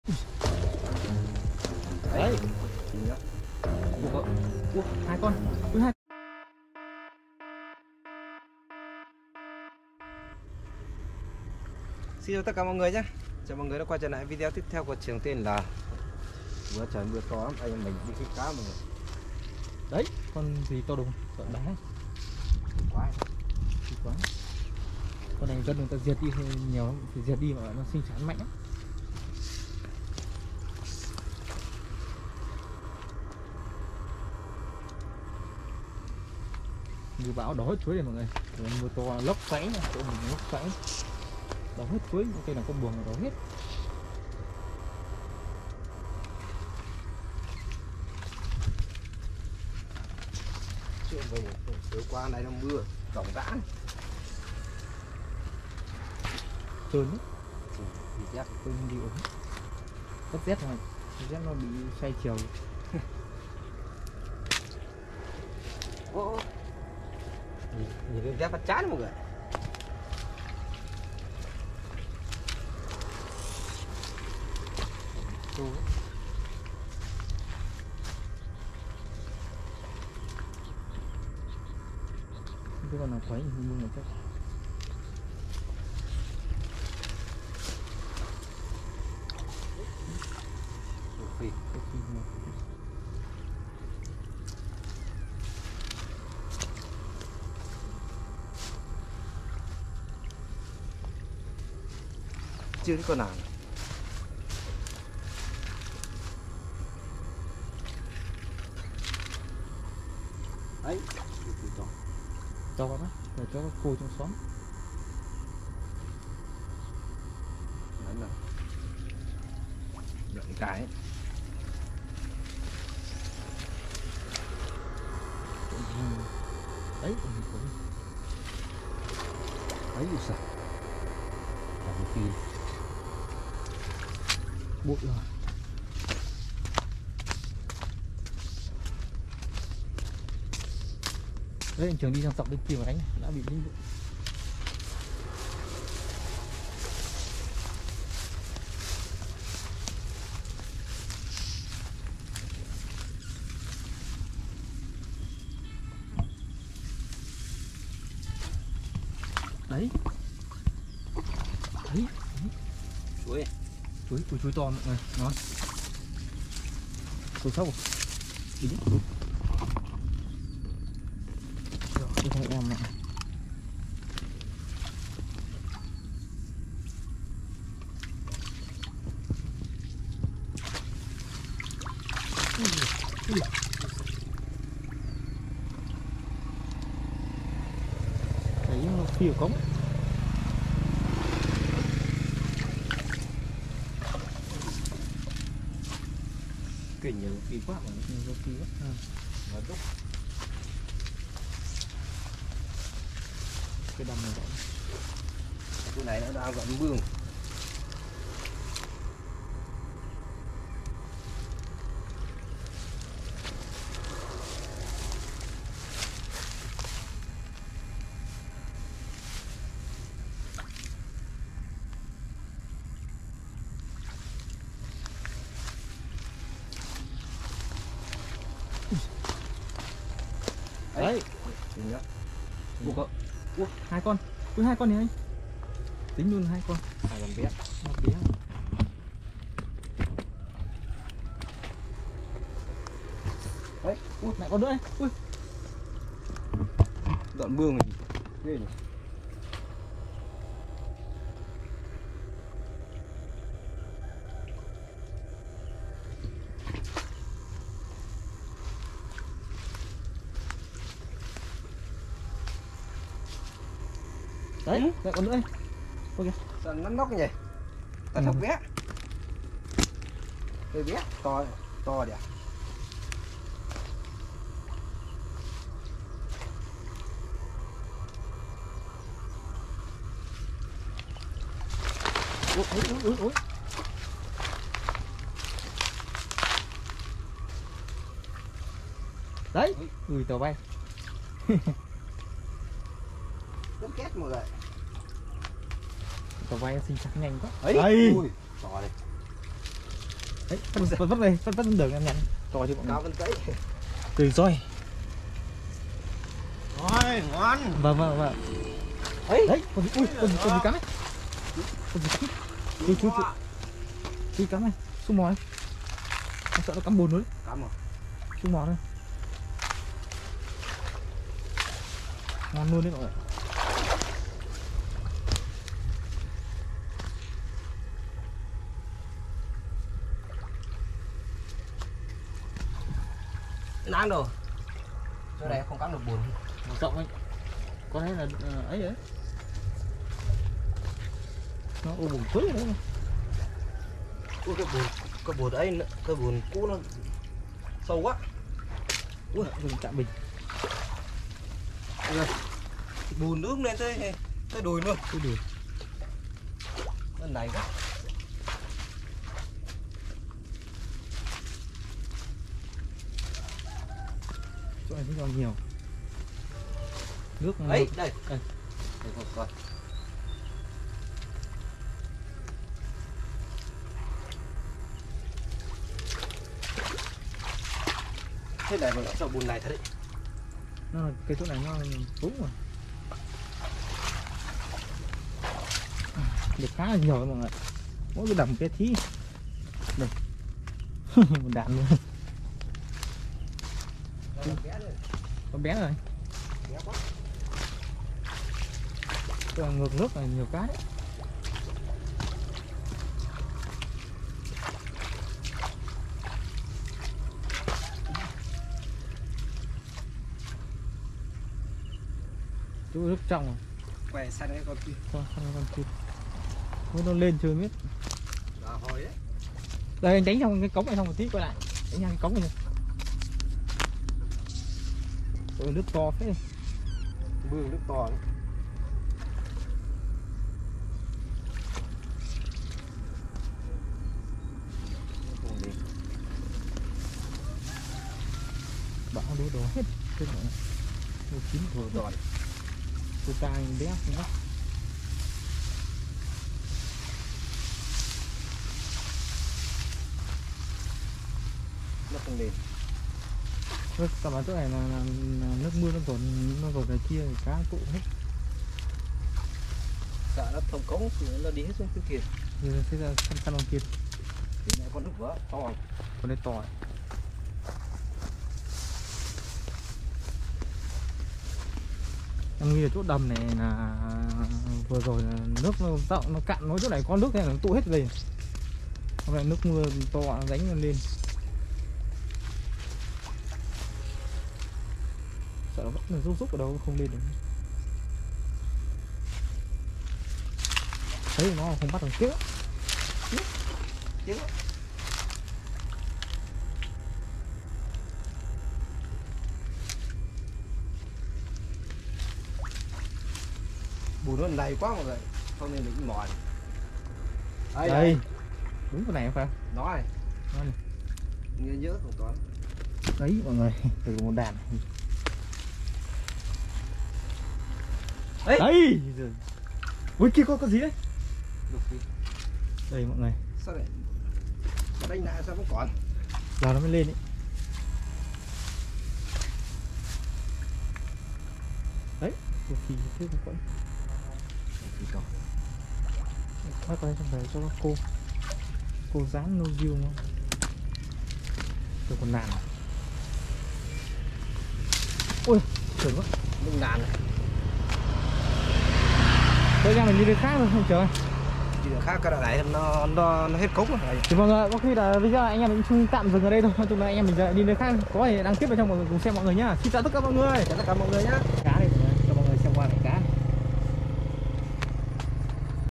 Kích Cá Ngày Mưa Bão Toàn Cá Lóc | Trường Tl
kíchcá cálóc trườngtl # kích cá ngày mưa bão | trường tl